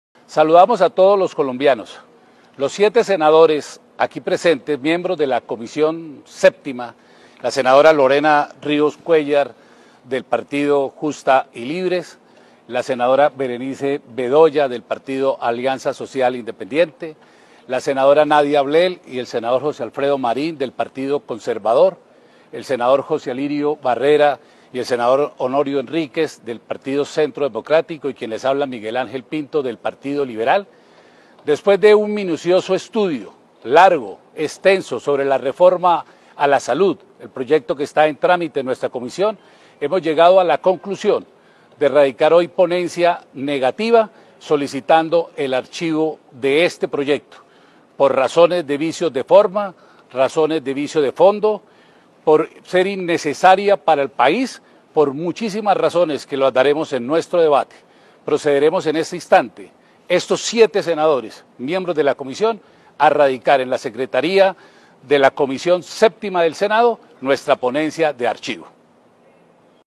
Se hundiría reforma a la salud por ponencia negativa de senadores del Centro Democrático.
Sé hundiría reforma a la salud. 7 senadores de la Comisión VII radicaron ponencia negativa a la reforma a la salud, considerándola como innecesaria y nefasta para el país.